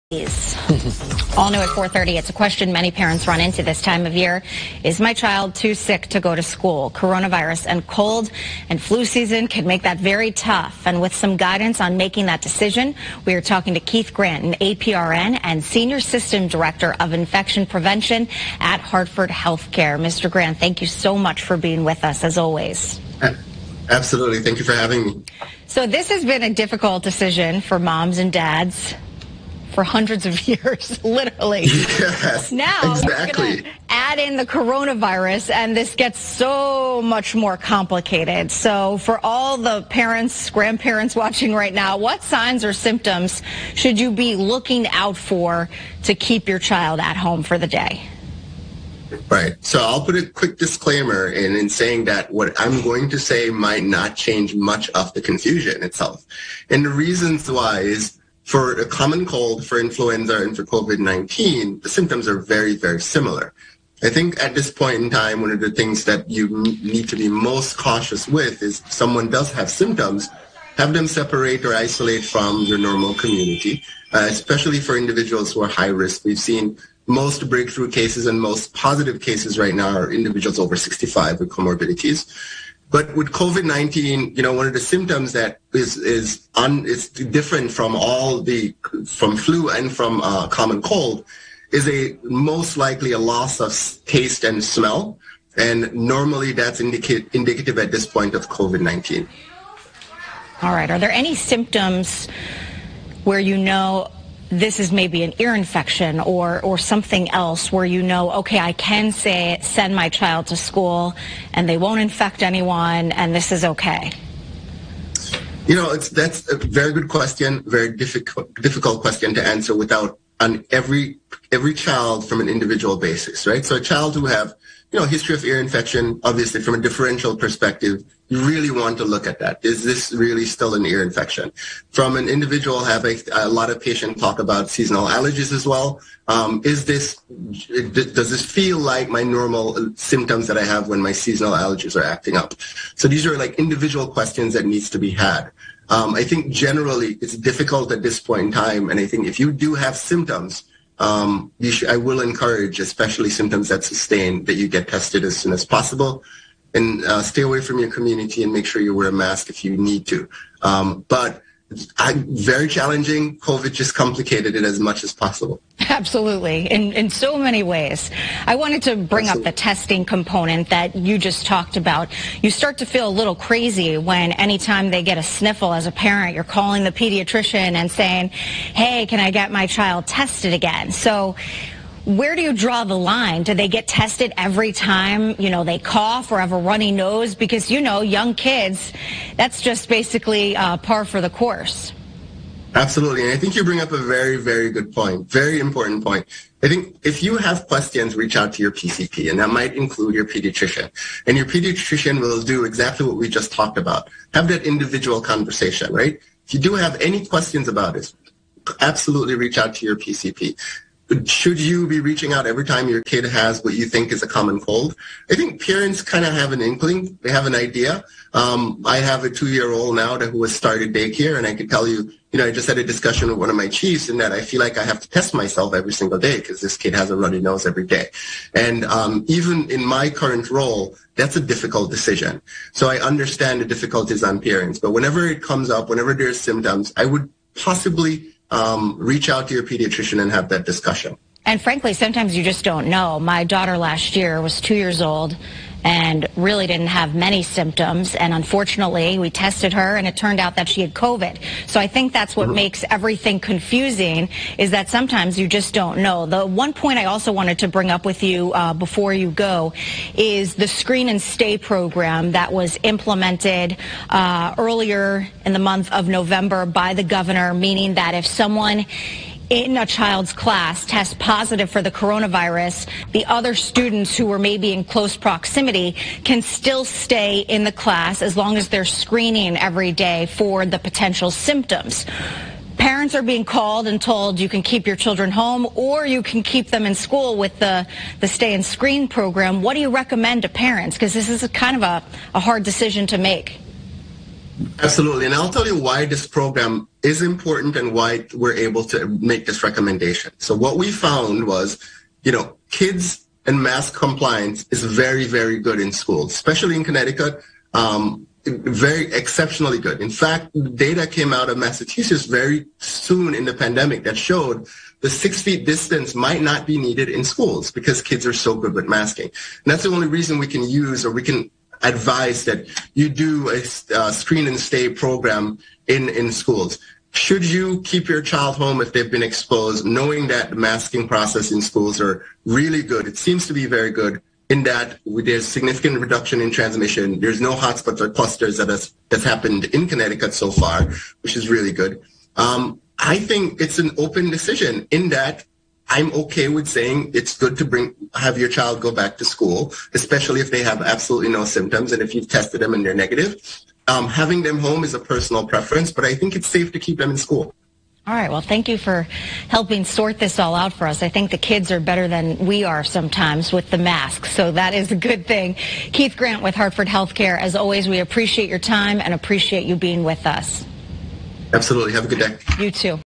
Differentiating COVID-19 Symptoms From the Common Cold
Keith Grant, APRN, Senior System Director, Infection Prevention, Hartford HealthCare, says that anyone who is symptomatic should avoid high risk groups such as people over 65, and the loss of taste or smell should be taken seriously as a common indicator of COVID-19.